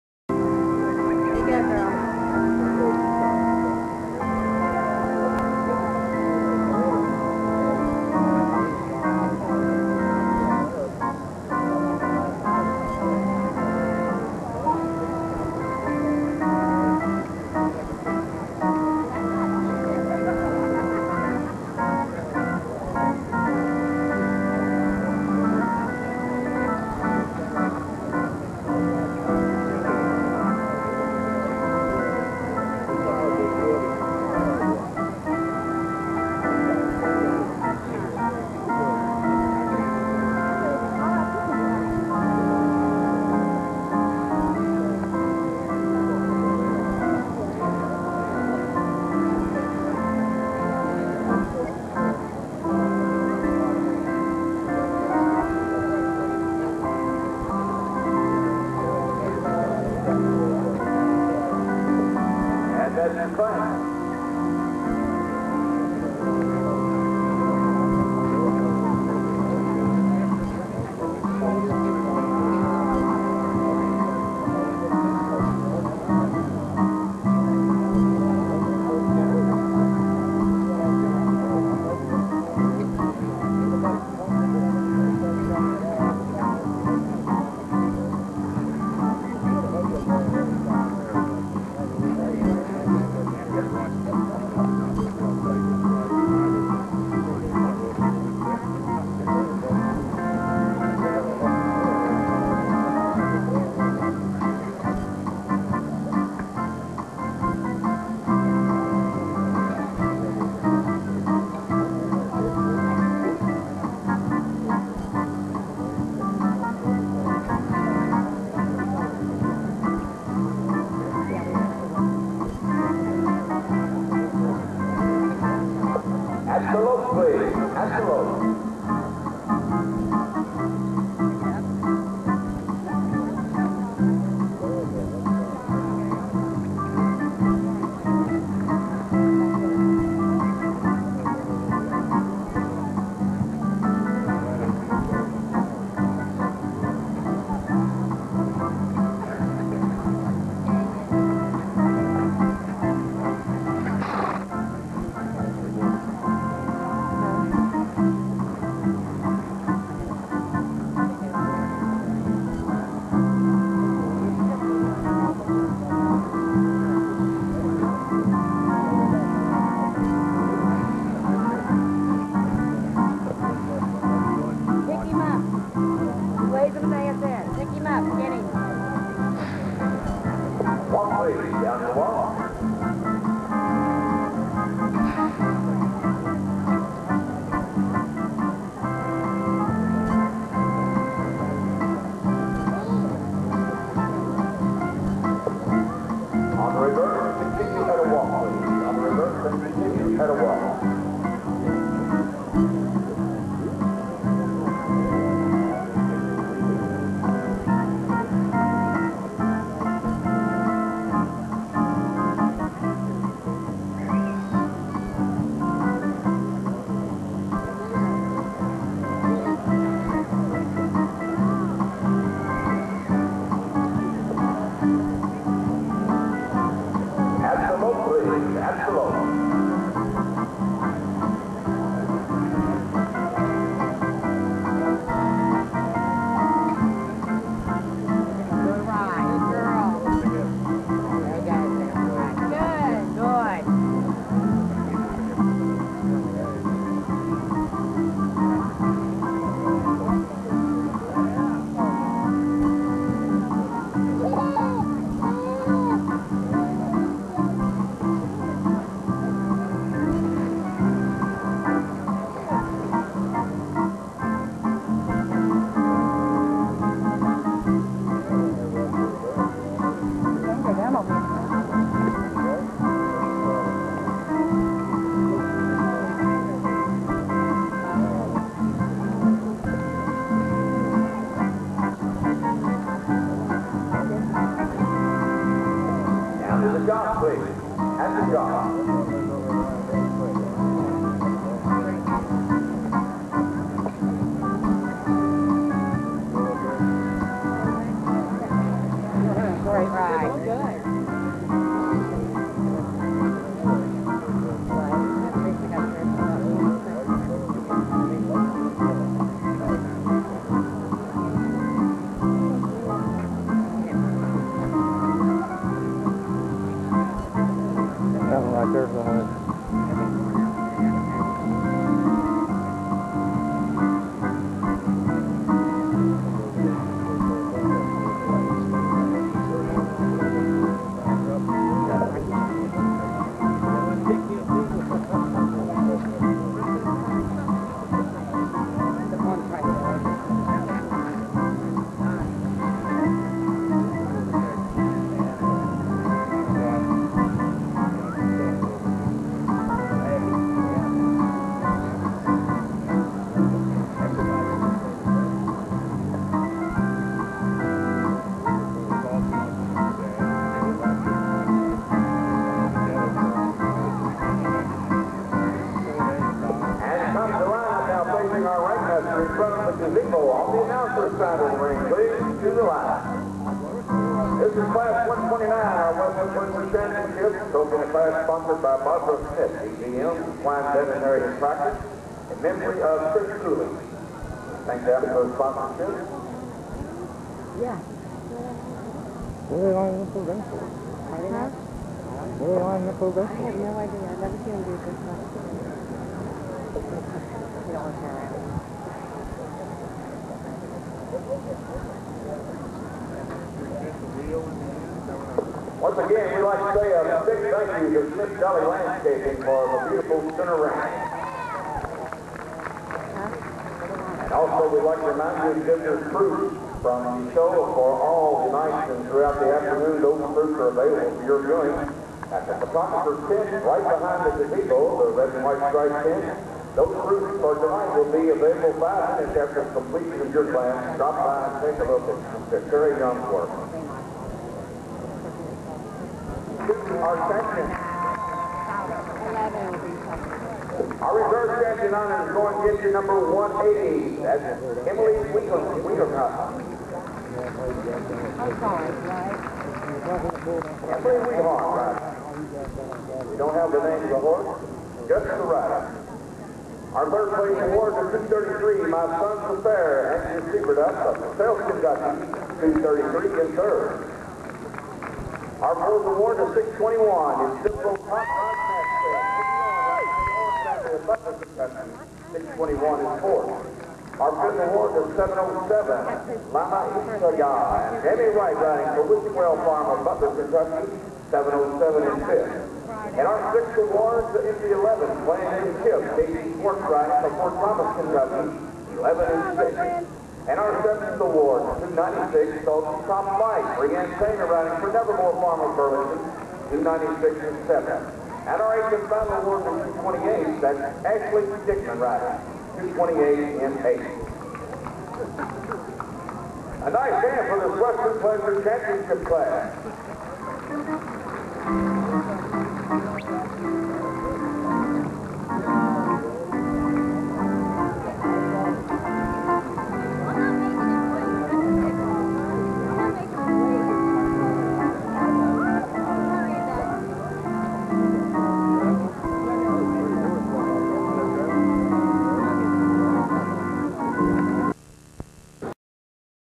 HotRod at the Fair!
Western Pleasure Champion ship Class.  He was worn out from 4 days of showing and his neck was the only thing loping the first way of the pen! LOL